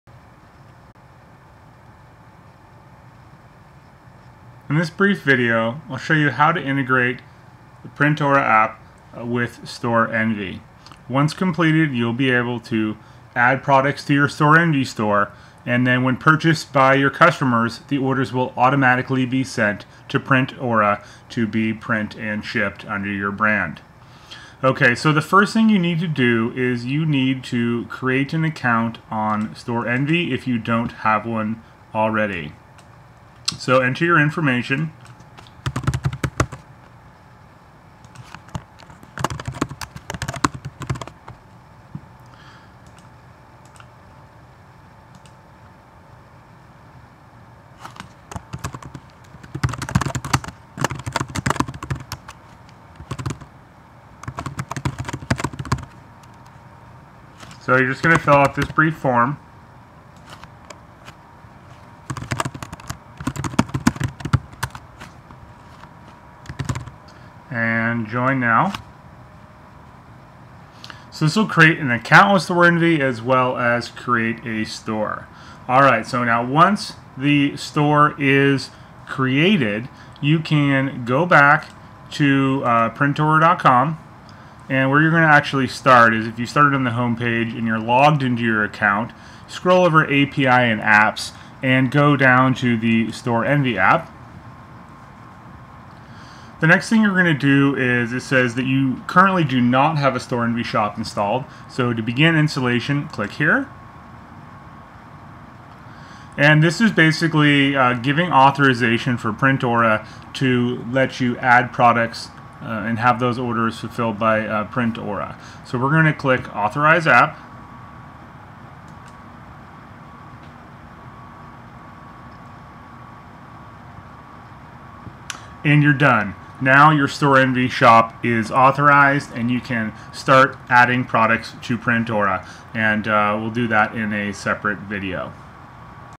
How to install PrintAura's Storenvy App
Brief video on how to install PrintAura's Storenvy app. Once installed you can add/edit products on PrintAura and have them display in your Storenvy shop. When those products are purchased by your customers the order details will automatically be sent to PrintAura who will print and ship them under YOUR BRAND. Easy t-shirt fulfillment with PrintAura and Storenvy.